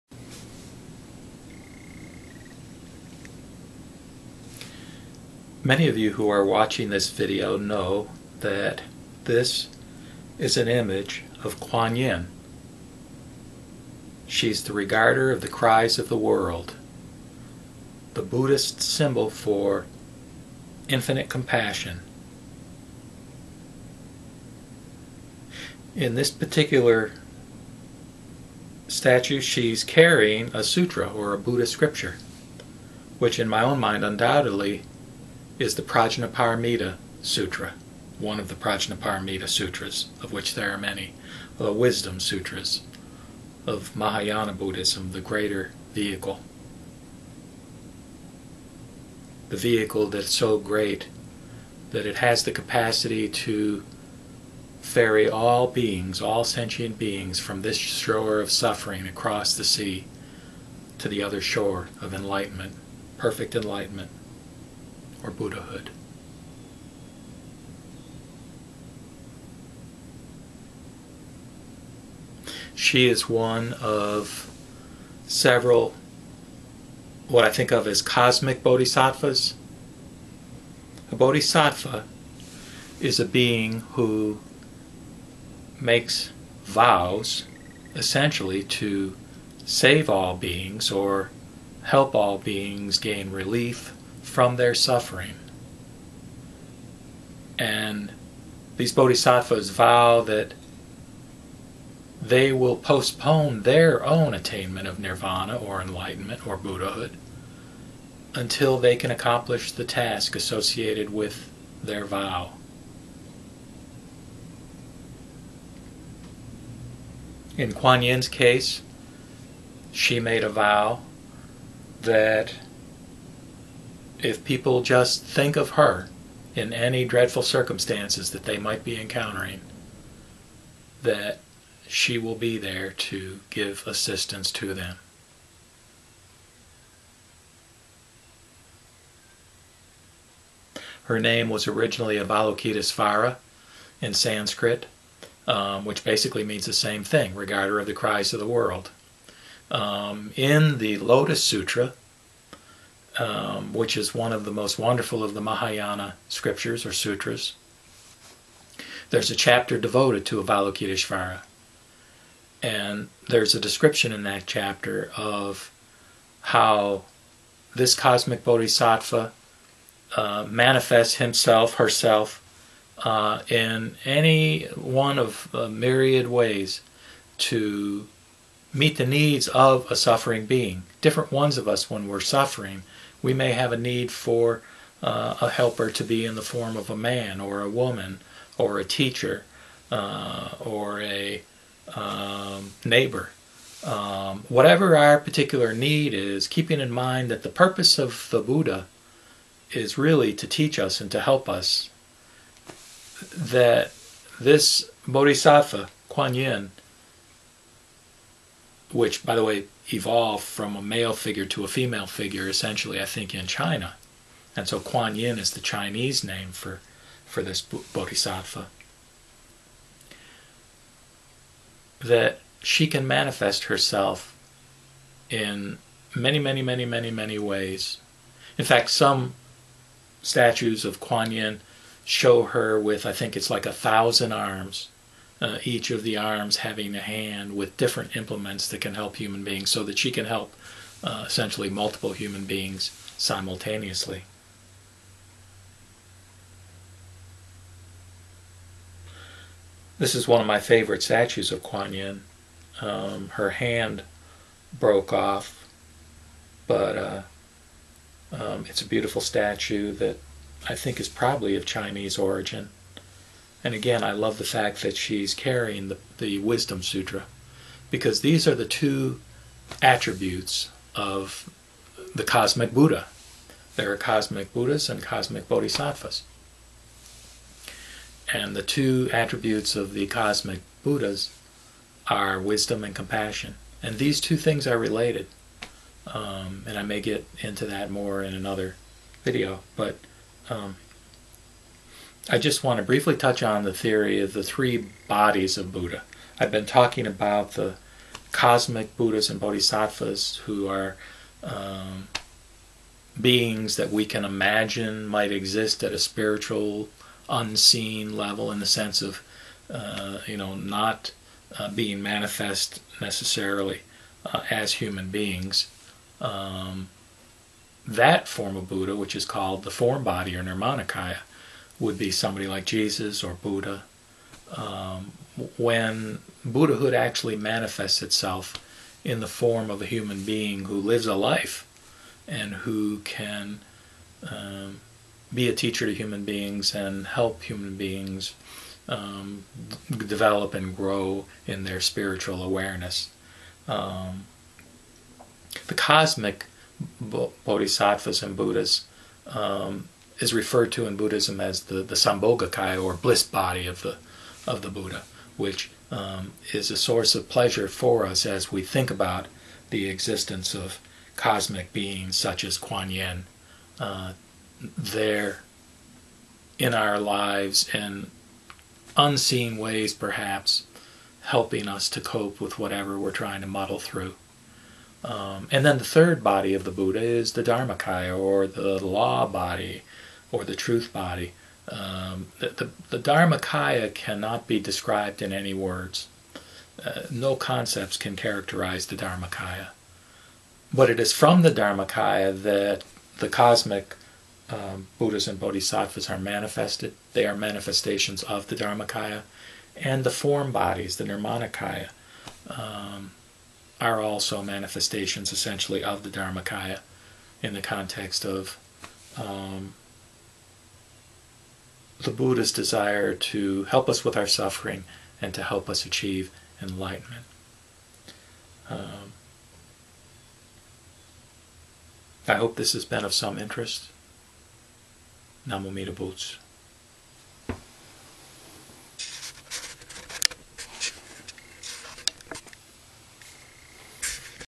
Kuan Yin: Bodhisattva of Infinite Compassion
Decribes (and shows a statue of) Kuan Yin, Regarder of the Cries of the World, a cosmic Bodhisattva within the Mahayana Buddhist tradition, and her Vow to help us beings meet the various difficulties and challenges we face in this life. Reference is also made to cosmic Buddhas. A brief explaination of the three Buddha body (Trikaya) theory is provided. The video ends with the Nembutsu, i.e., Namuamidabuts.